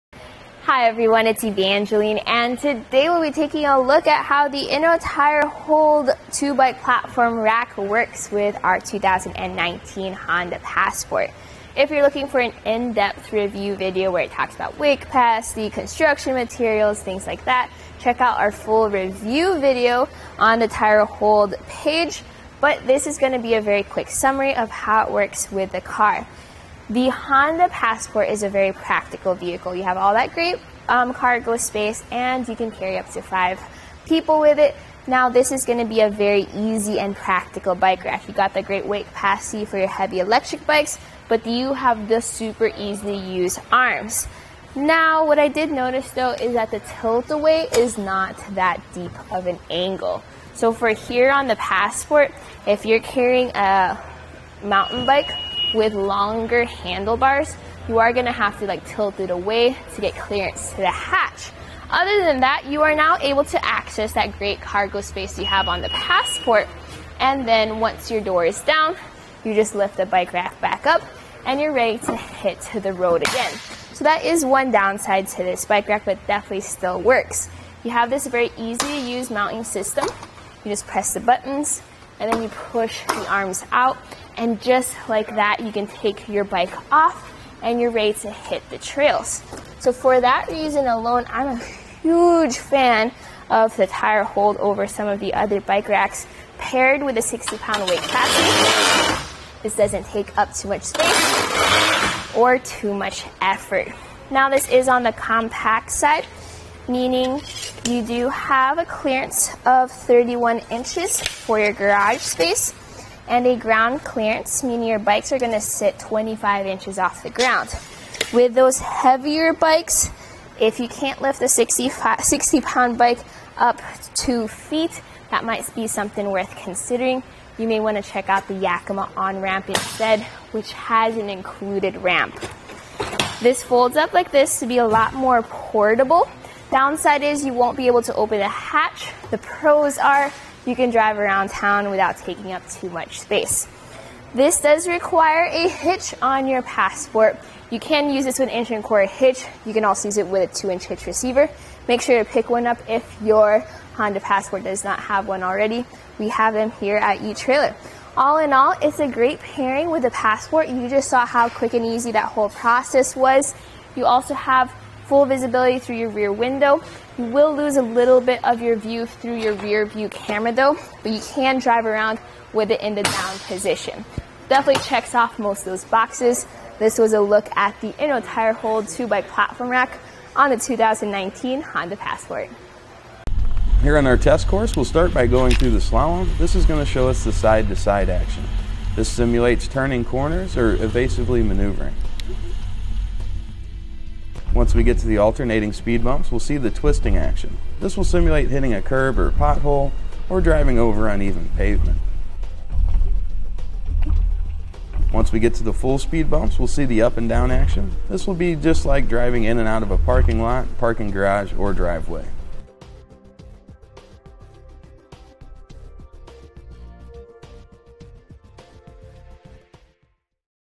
etrailer | Test Fit: Inno Tire Hold Bike Rack for 2 Bikes on a 2019 Honda Passport
Hi, everyone. It's Evangeline, and today we'll be taking a look at how the Inno Tire Hold 2-Bike Platform Rack works with our 2019 Honda Passport. If you're looking for an in-depth review video where it talks about weight capacity, construction materials, things like that, check out our full review video on the tire hold page. But this is gonna be a very quick summary of how it works with a car. The Honda Passport is a very practical vehicle. You have all that great cargo space, and you can carry up to five people with it.

Now this is gonna be a very easy and practical bike rack. You got the great weight capacity for your heavy electric bikes but you have the super easy use arms. Now, what I did notice though, is that the tilt away is not that deep of an angle. So for here on the Passport, if you're carrying a mountain bike with longer handlebars, you are gonna have to like tilt it away to get clearance to the hatch. Other than that, you are now able to access that great cargo space you have on the Passport.

And then once your door is down, you just lift the bike rack back up, and you're ready to hit to the road again. So that is one downside to this bike rack, but definitely still works. You have this very easy use mounting system. You just press the buttons and then you push the arms out, and just like that, you can take your bike off and you're ready to hit the trails. So for that reason alone, I'm a huge fan of the Tire Hold over some of the other bike racks paired with a 60 pound weight capacity.

This doesn't take up too much space or too much effort. Now this is on the compact side, meaning you do have a clearance of 31 inches for your garage space and a ground clearance, meaning your bikes are gonna sit 25 inches off the ground. With those heavier bikes, if you can't lift the 60 pound bike up two feet that might be something worth considering. You may wanna check out the Yakima OnRamp instead which has an included ramp. This folds up like this to be a lot more portable.

Downside is you won't be able to open a hatch. The pros are, you can drive around town without taking up too much space. This does require a hitch on your Passport. You can use a 28 inch and core hitch. You can also use it with a two inch hitch receiver. Make sure you pick one up, if your Honda Passport does not have one already we have them here at etrailer. All in all, it's a great pairing with a Passport. You just saw how quick and easy that whole process was. You also have full visibility through your rear window. You will lose a little bit of your view through your rear view camera though, but you can drive around with it in the down position. Definitely checks off most of those boxes. This was a look at the Inno Tire Hold 2-Bike Platform Rack on a 2019 Honda Passport. Narrator Here on our test course we'll start by going through the slalom. This is gonna show us the side to side action. This simulates turning corners or evasively maneuvering. Once we get to the alternating speed bumps, we'll see the twisting action. This will simulate hitting a curb or pothole or driving over uneven pavement. Once we get to the full speed bumps, we'll see the up and down action. This will be just like driving in and out of a parking lot, parking garage or driveway..